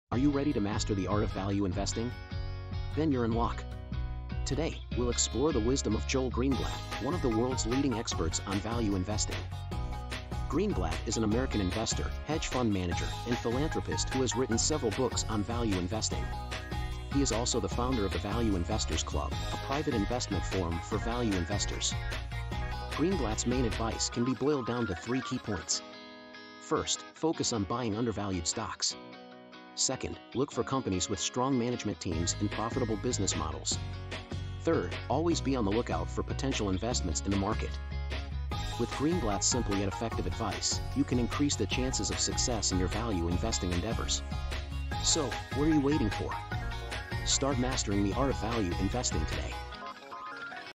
Mastering the Art of Value Investing: Joel Greenblatt's Wisdom 📈💼
In this informative video, we delve into the profound wisdom of Joel Greenblatt, one of the world's leading experts on value investing. Join us as we explore Greenblatt's simple yet highly effective advice on how to become a successful value investor.

Value investing is an investment strategy that aims to identify undervalued stocks and assets in the market. Greenblatt's expertise and track record in this field have earned him a reputation as a master investor. In this video, we uncover the core principles and strategies that have contributed to his remarkable success.

Discover how Greenblatt's approach combines fundamental analysis and a long-term investment horizon. Learn how to assess a company's intrinsic value, identify market inefficiencies, and make informed investment decisions. We break down his step-by-step process and share practical tips for implementing his value investing philosophy.

Whether you're a novice investor or an experienced one looking to refine your strategy, this video provides valuable insights into the art of value investing. Gain a deeper understanding of the principles that have shaped Greenblatt's investment philosophy and unlock the potential to generate consistent returns.

Join us on this educational journey as we uncover the secrets to successful value investing, guided by the wisdom of Joel Greenblatt. Don't miss out on this opportunity to enhance your investing skills and achieve financial success.